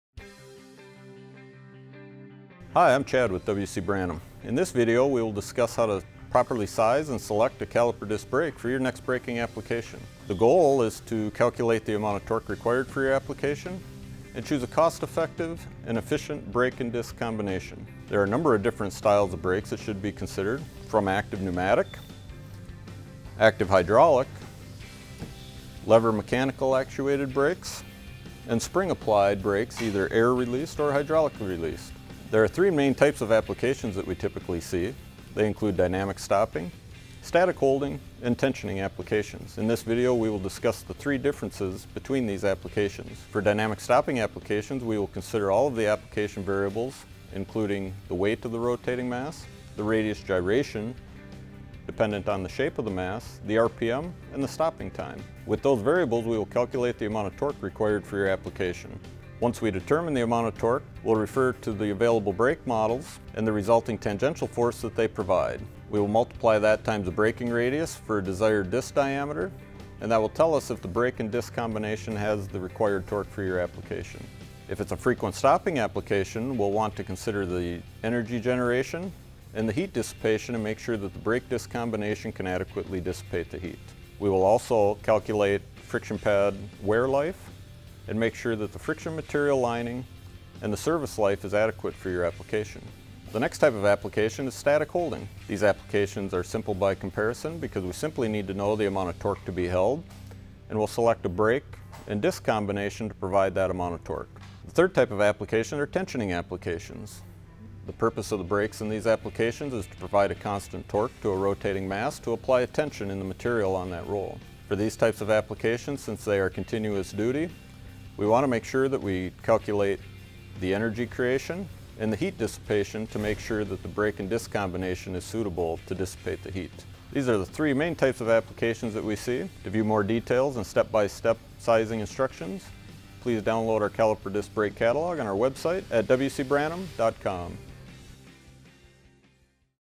Caliper Disc Brake Sizing: How to Properly Size and Select a Caliper Disc Brake
From active pneumatic and active hydraulic to lever mechanical actuated and spring applied brakes, you can customize your application with the perfect product. Each application type requires a different type of caliper disc brakes and features, so we’ll help you narrow down what product you’ll need based on the application you’re using it for.